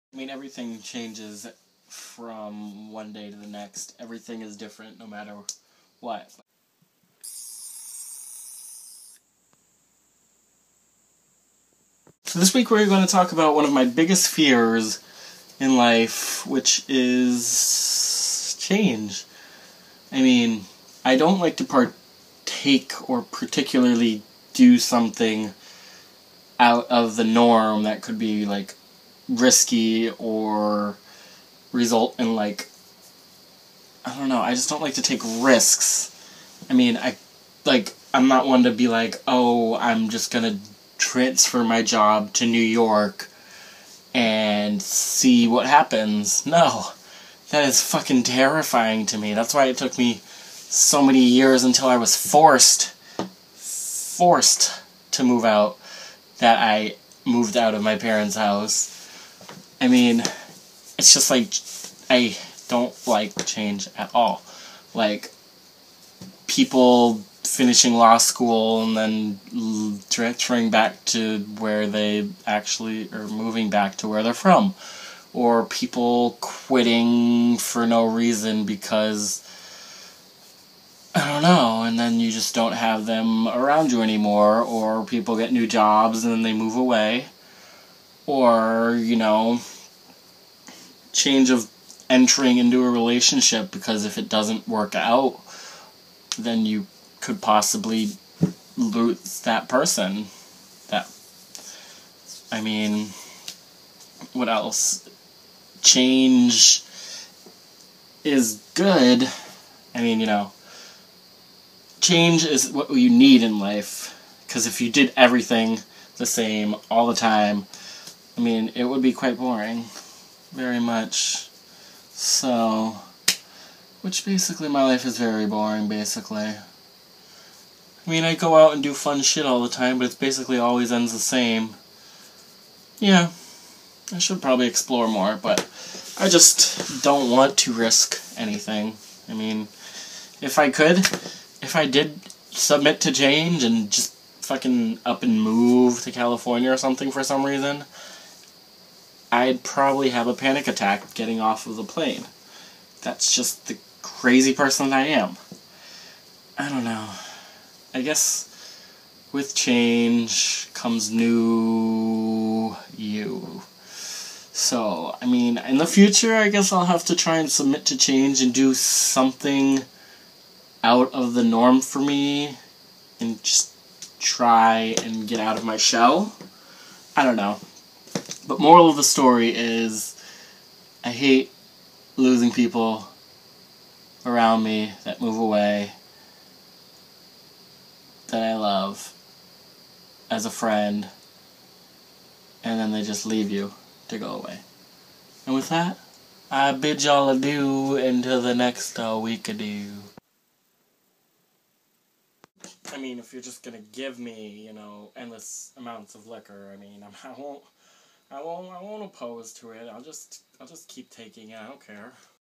Ricky Bobby Talks About: Change
This week Ricky bobby talks about change and how afraid he is to make some change and how much he hates change around him, even though with change comes a new you.